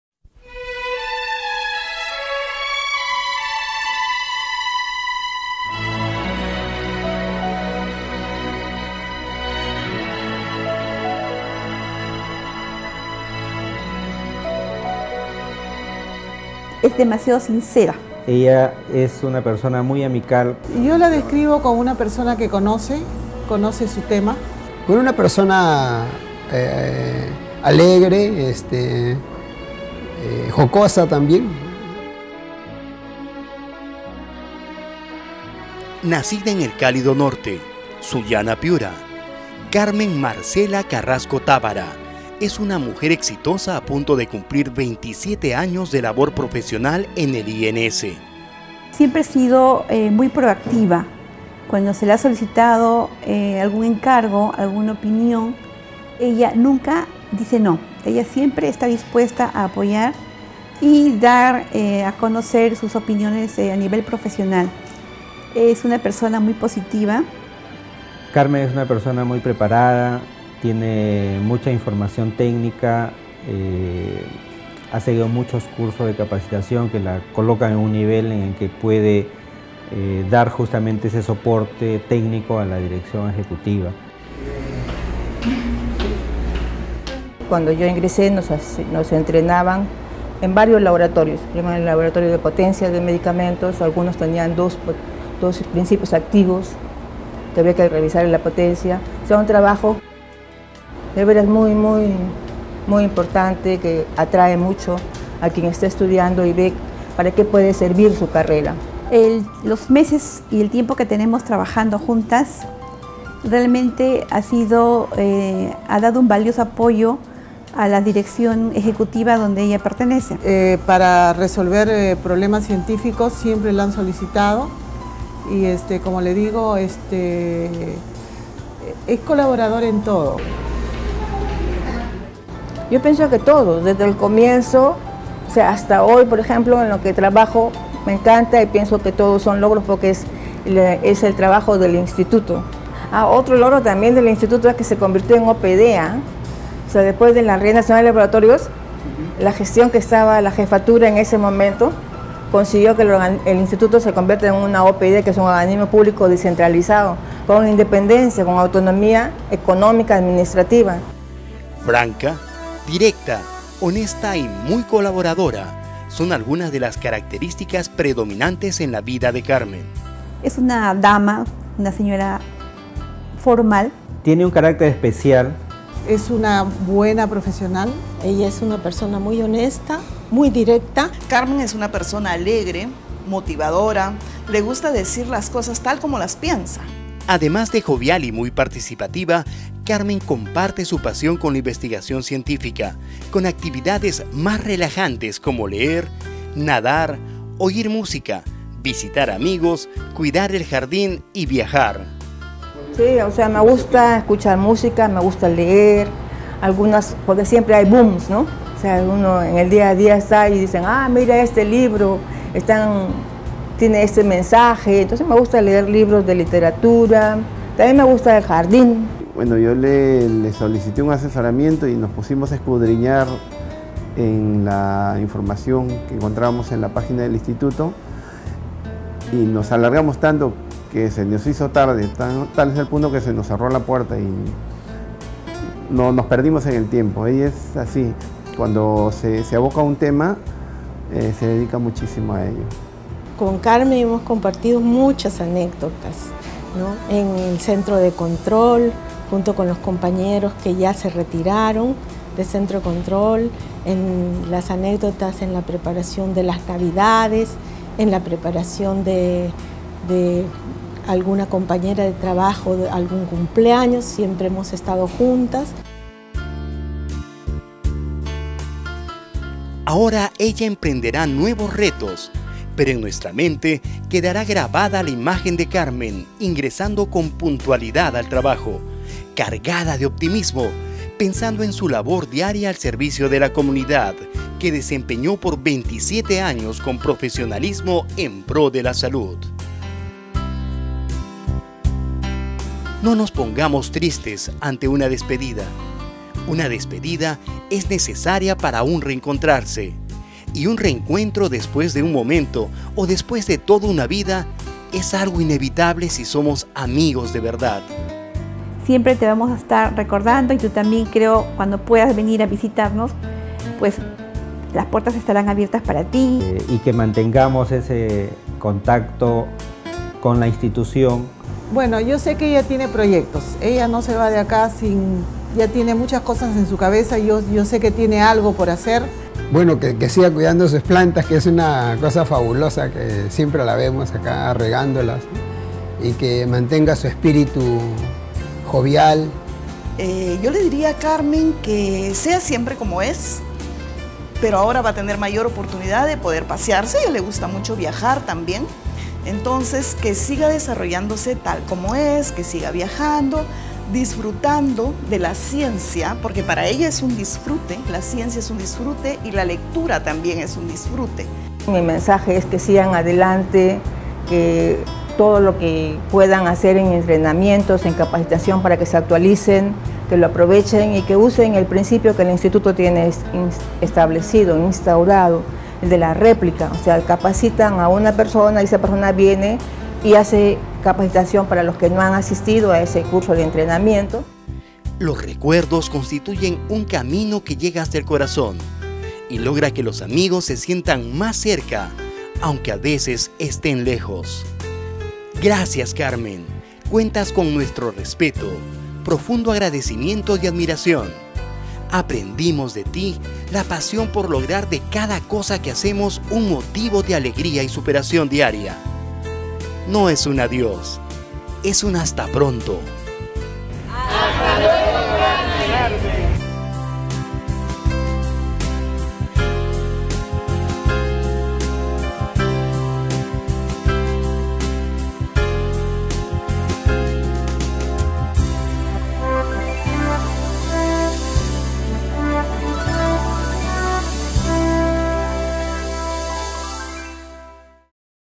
Reconocimiento a la Bióloga Carmen Carrasco Távara
Al cumplir 27 años ininterrumpidos de labor, en las diversas sedes del INS, la Bióloga Carmen Carrasco Távara fue homenajeada en ceremonia protocolar realizado en el Auditorio del Centro Nacional de Alimentación y Nutrición (CENAN) del Instituto Nacional de Salud.